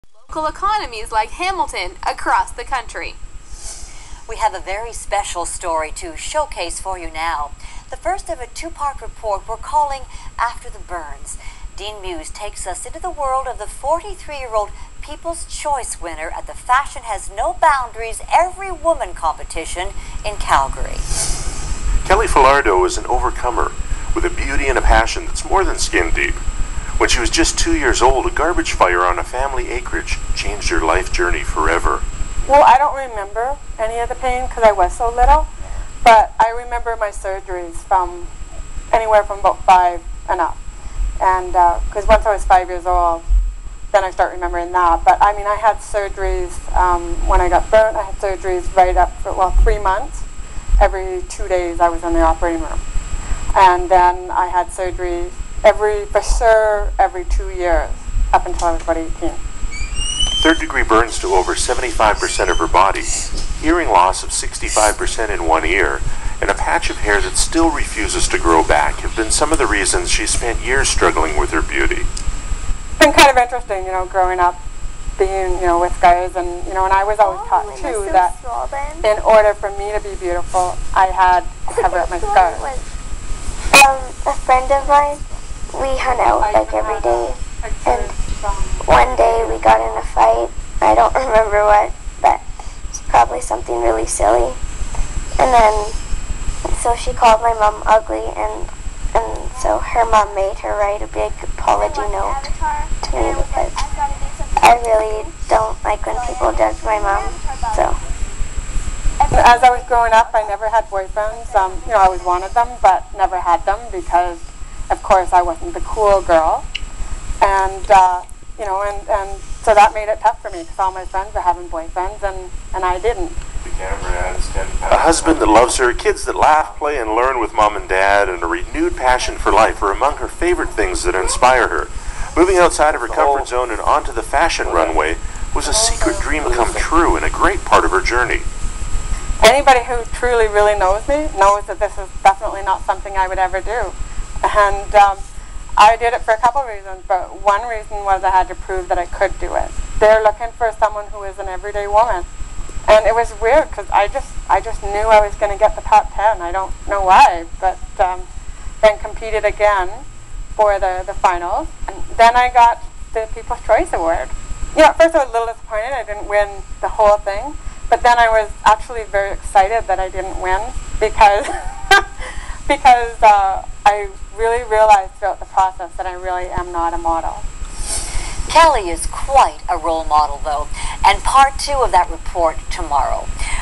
CTS TV Interview of Kelly Falardeau - After the Burn - Part 1
Footage compliments of CTS TV - Edmonton This video clip is about Kelly Falardeau, a burn survivor who enters a beauty pageant and wins the Peoples' Choice Award.